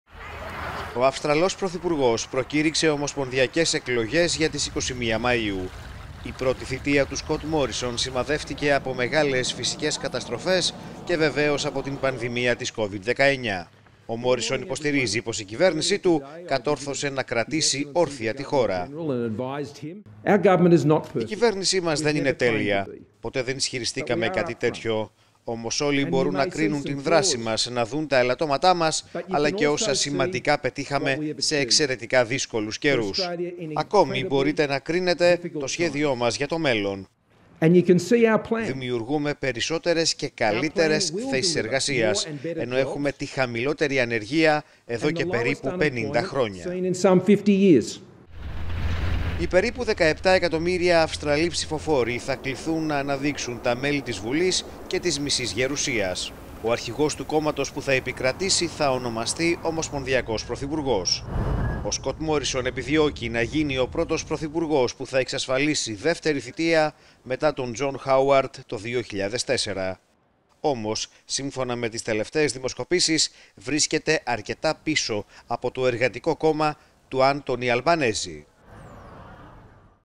Αυστραλία: Εκλογές στις 21 Μαΐου προκήρυξε ο Σκοτ Μόρισον
Οι περίπου 17 εκατομμύρια Αυστραλοί ψηφοφόροι θα κληθούν να αναδείξουν τα μέλη της Βουλής και της μισής Γερουσίας